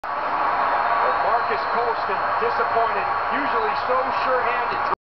Marques Colston is a dick.
This video proves what a dick Marques Colston is, after giving up a fumble, he slams his helmet and pushes his teammate, Benjamin Watson, who was trying to console Colston.